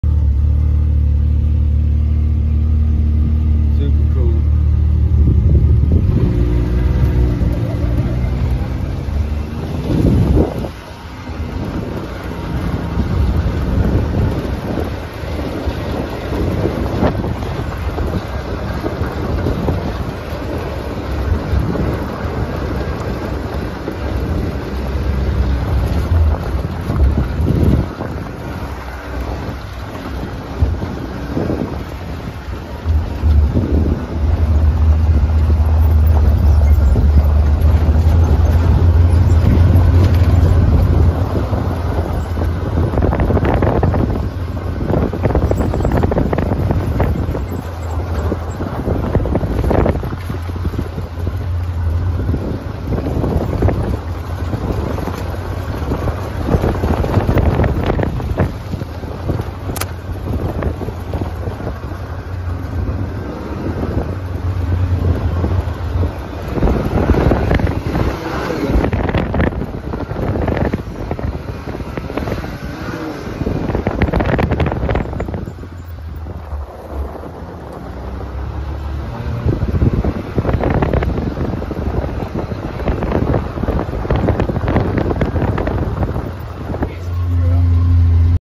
x3 side by side at the mountain top at Liberty
Take a ride from the mountain top at Liberty, Wa in the X3 with Off Camber Powersports owner Scott Wynne and myself. Enjoy!